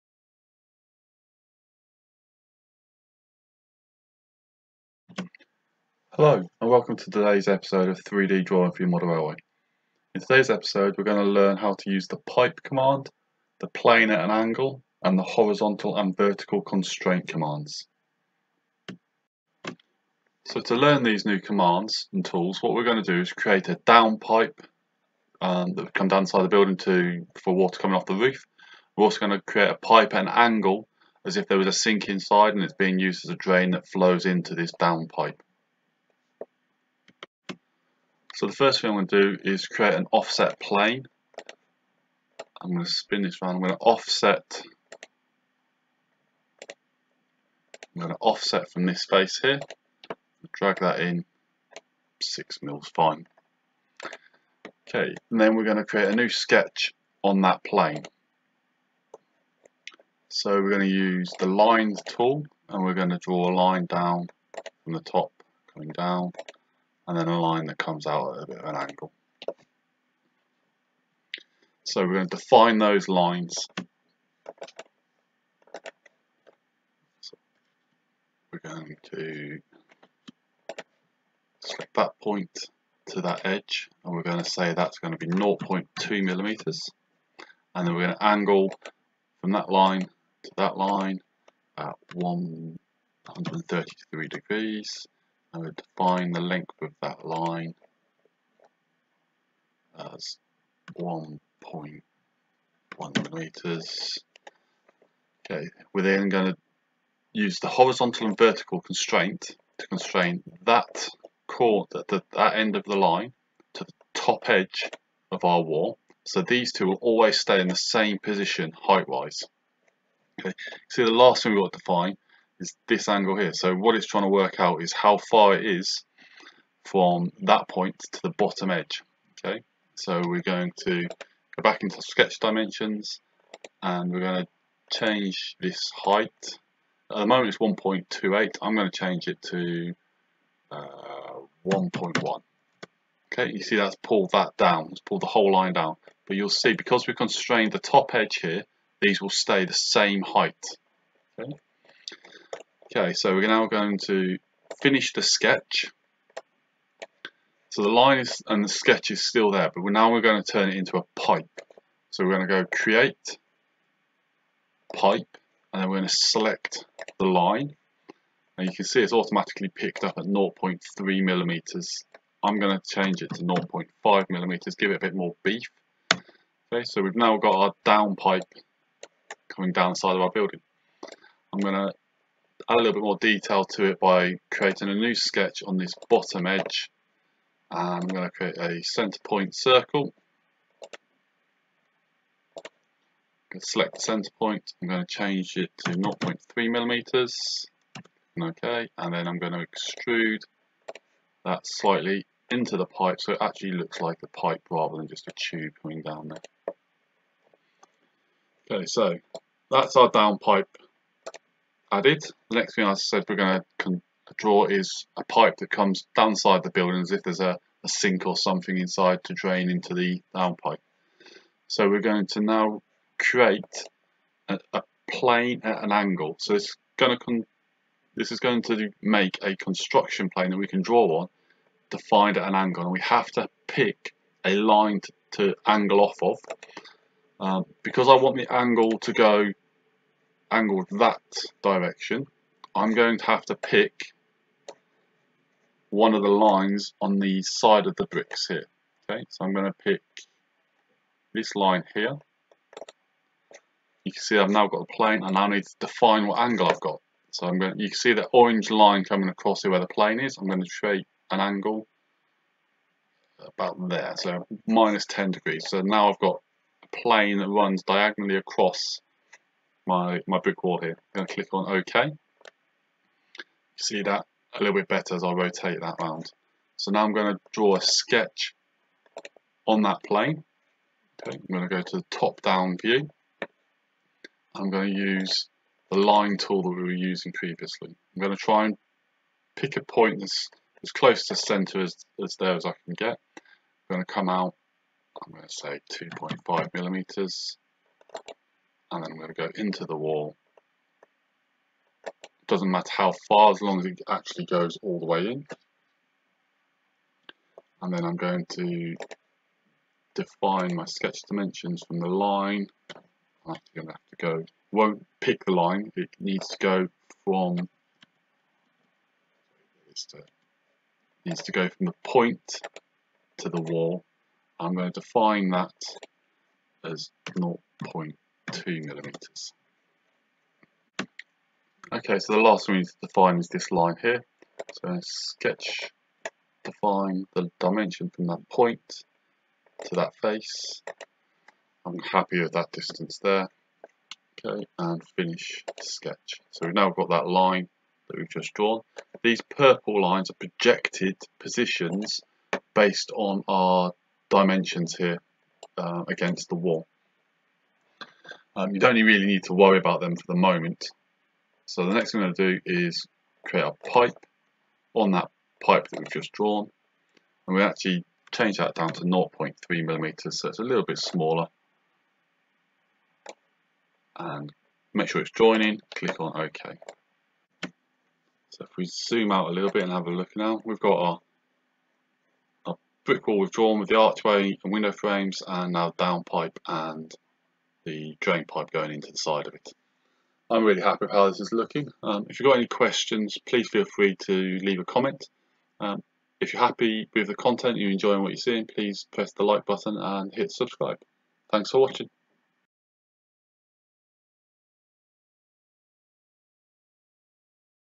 Lesson 11 - Fusion 360 Pipe and Plane at an Angle tutorial
Lesson 11

A series of tutorials teaching you how to use Fusion 360 to be able to draw items ready to 3D print for your model railway.

In todays lesson we draw rain pipes and continue our series of videos that lead to us drawing a small building using the following new tools and commands:

Pipe
Plane at Angle

In the next lesson we will copy the body of our wall and start to turn it into a building shape.

Anycubic Photon Original
Replacement FEP
1L White Resin
UV Curing lamp
Isopropyl Alcohol
Nitrile Gloves